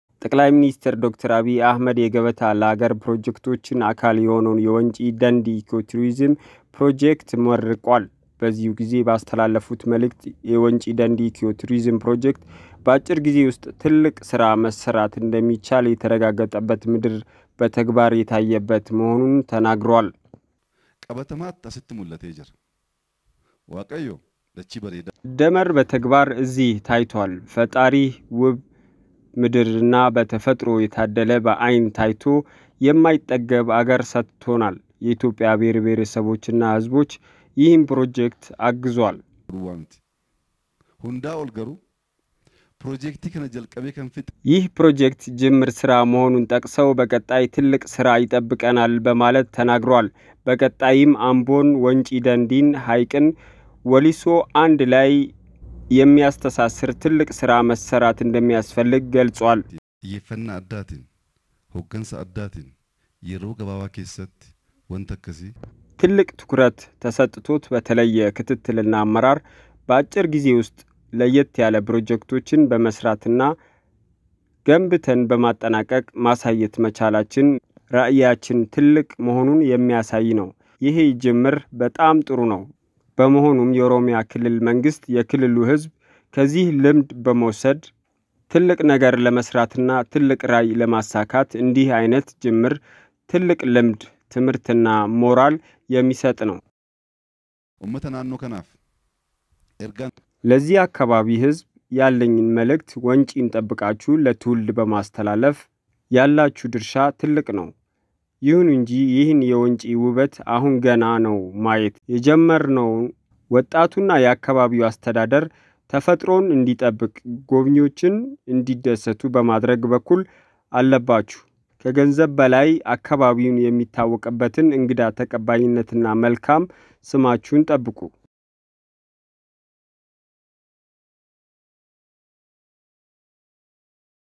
Wenci Dendi project ጠቅላይ ሚኒስትር ዐቢይ አሕመድ የገበታ ለአገር ፕፎጀክቶች አካል የሆነውን የወንጪ-ደንዲ ኢኮቱሪዝም ፕሮጀክት መርቀዋል።
ጠቅላይ ሚኒስትር ዐቢይ አሕመድ የገበታ ለአገር ፕፎጀክቶች አካል የሆነውን የወንጪ-ደንዲ ኢኮቱሪዝም ፕሮጀክት መርቀዋል።